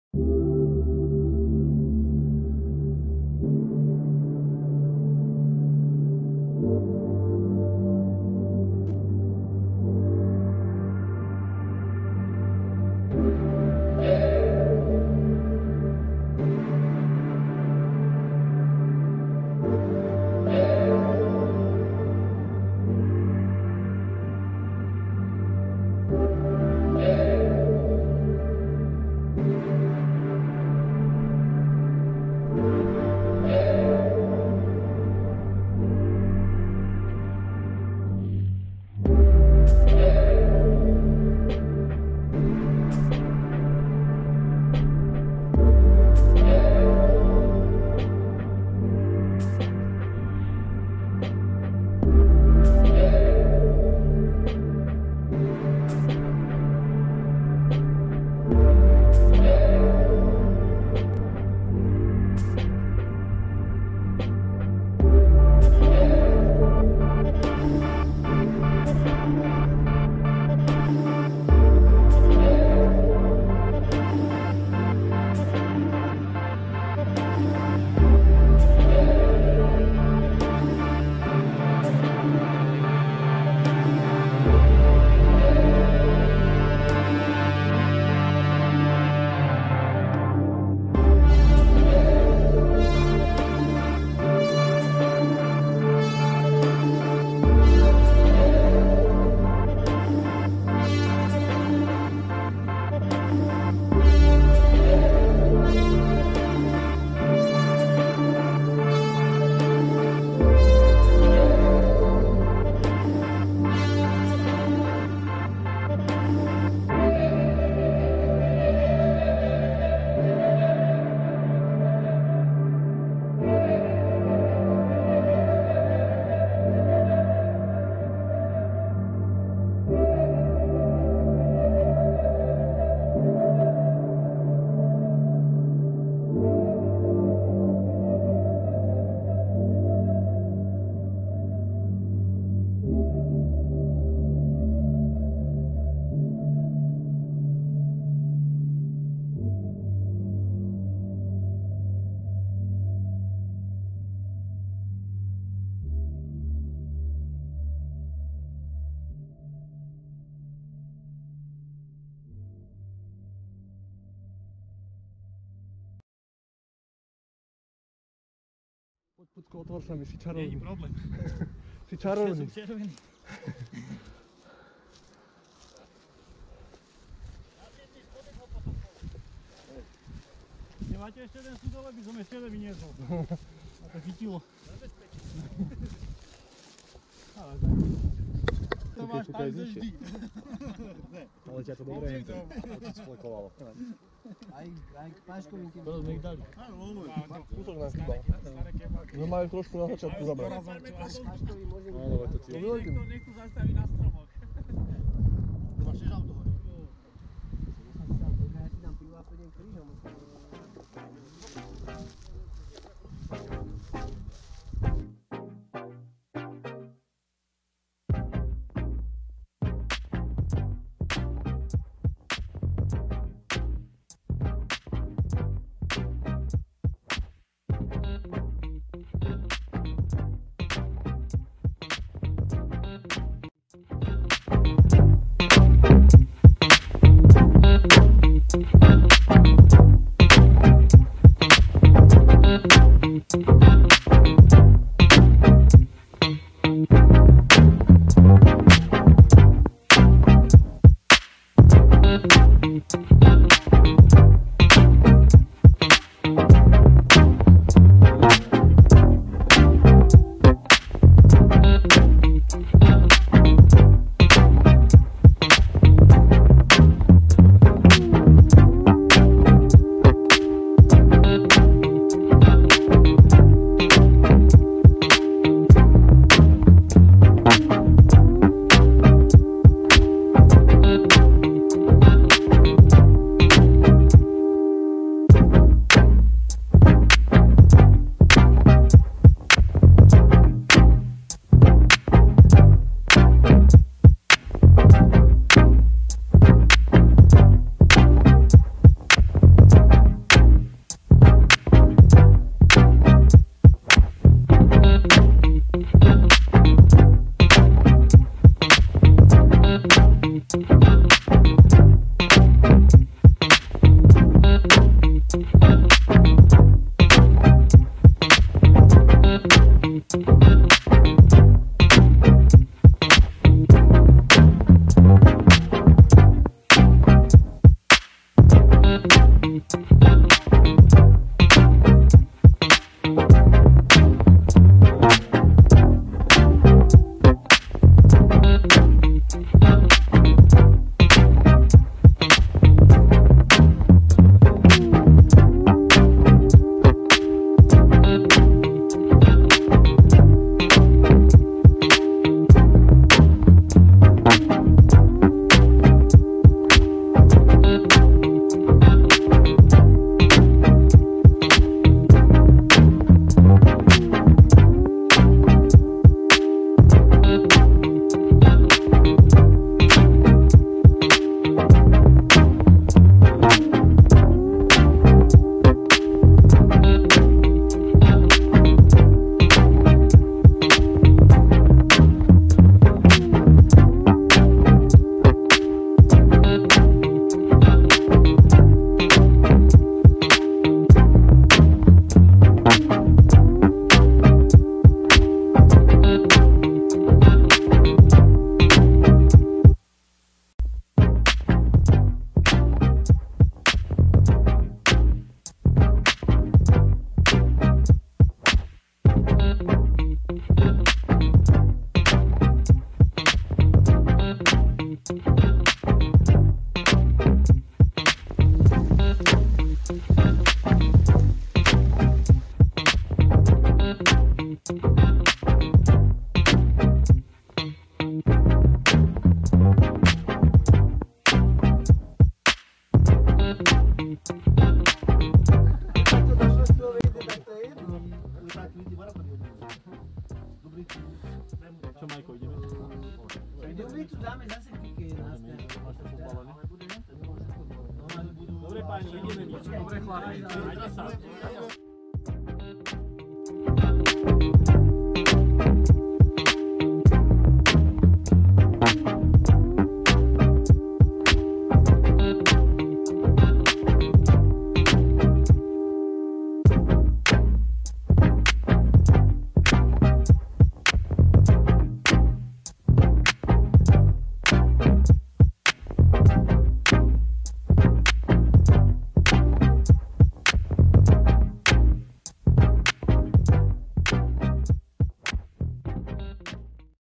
Cestou necestou na Strážov / GOPRO 6 /GOPRO KARMA
Ťažká a zároveň krásna cesta na Strážovské vrchy s 50l sudom piva na chrbte a skvelou partiou.Video je nahrávané s GOPRO 6, GOPRO GRIP a GOPRO KARMA.